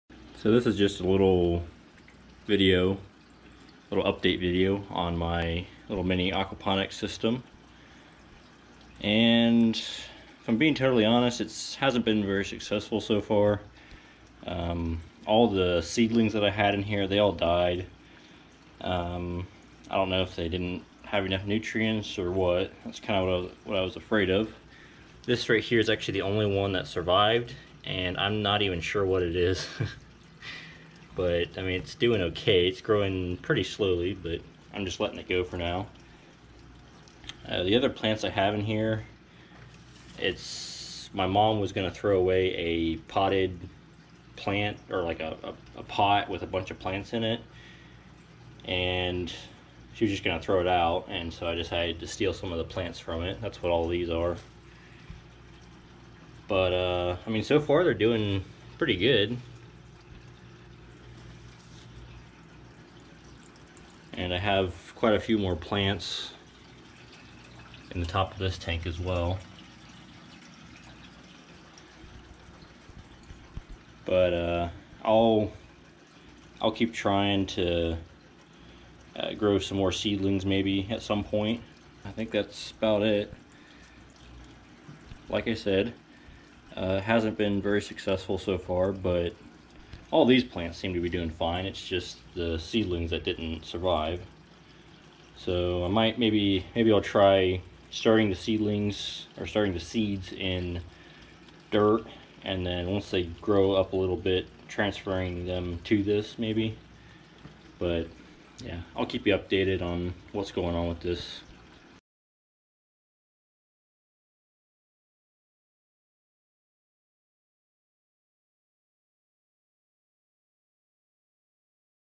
Mini Aquaponics System Update
I hadn't though about it for a while, but all of a sudden I realized that I hadn't done an update on the mini aquaponic system since I posted the first video on it, so here it is.